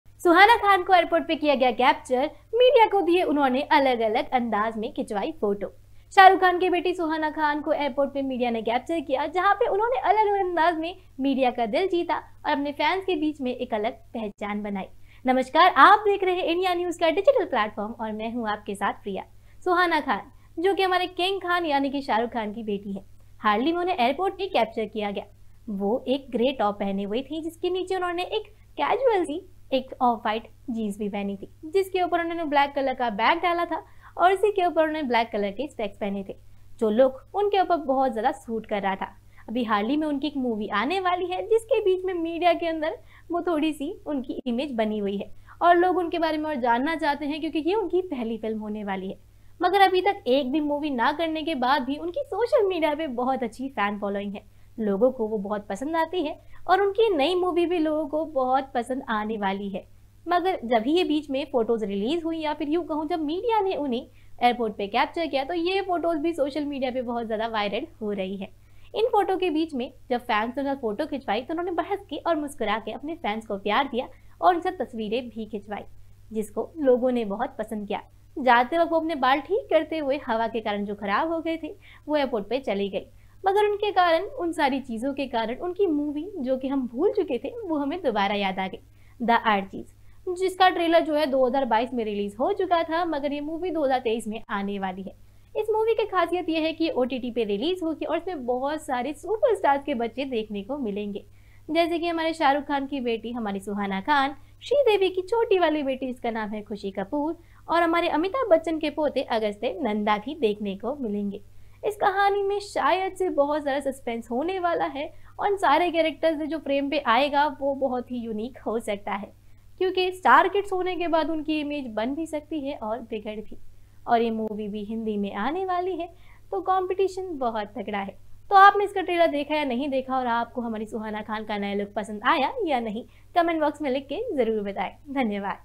Suhana Khan ने पहली बार Fans और Reporters के साथ किया ऐसा कारनामा? | Fans से मिला प्यार
Suhana Khan ने पहली बार Fans और Reporters के साथ किया ऐसा कारनामा? वीडियो देख Shah Rukh Khan को
होगा गर्व